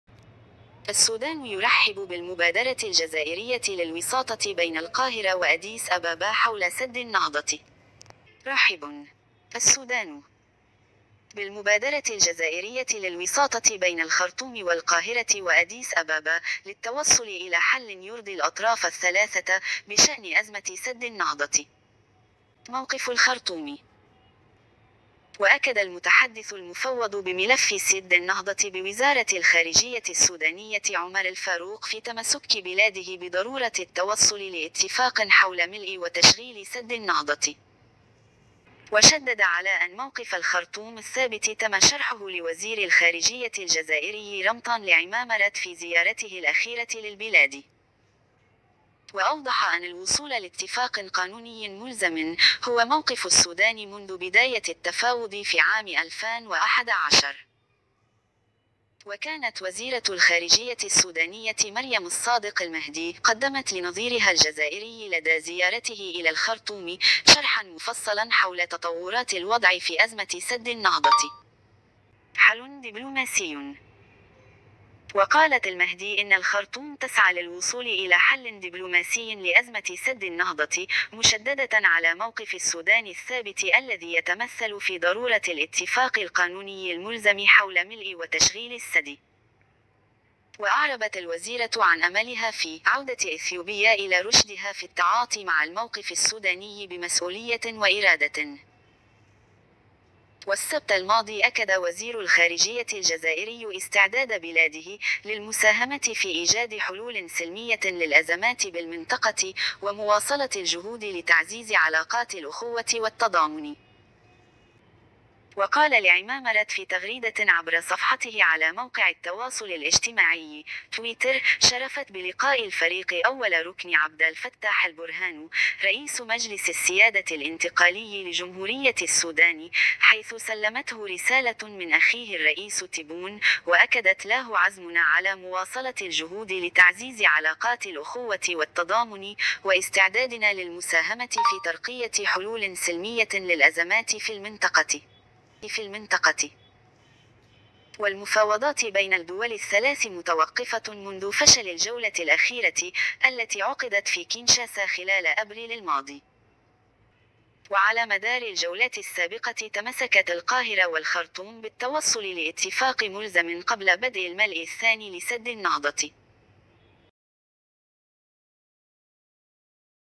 السودان يرحب بالمبادرة الجزائرية للوساطة بين القاهرة وأديس أبابا حول سد النهضة
رحب السودان، بالمبادرة الجزائرية للوساطة بين الخرطوم والقاهرة وأديس أبابا، للتوصل إلى حل يرضي الأطراف الثلاثة، بشأن أزمة سد النهضة.

موقف الخرطوم

وأكد المتحدث المفوض بملف سدّ النهضة بوزارة الخارجية السودانية عمر الفاروق في تمسك بلاده بضرورة التوصل لاتفاق حول ملء وتشغيل سد النهضة.